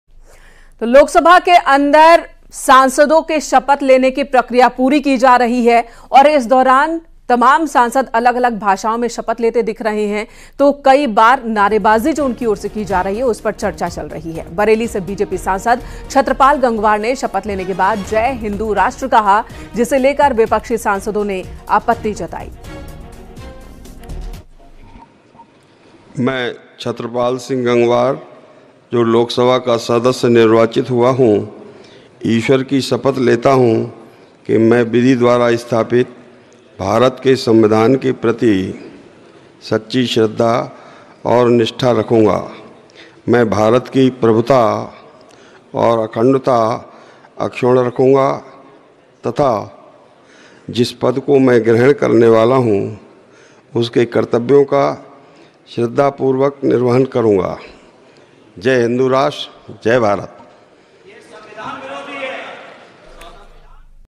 Parliament Session 2024 : छत्रपाल गंगवार के 'जय हिंद' का नारा लगाते ही विपक्ष ने... | Top News
18वीं लोकसभा के लिए सोमवार से ही निर्वाचित सदस्यों का शपथ ग्रहण कार्यक्रम जारी है। इस बीच, उत्तर प्रदेश के बरेली से बीजेपी सांसद छत्रपाल सिंह गंगवार ने शपथ के बाद "जय हिंदू राष्ट्र, जय भारत" बोल दिया। इसके बाद संसद में हंगामा मच गया। उनकी शपथ पर विपक्षी नेताओं ने आपत्ति जताई।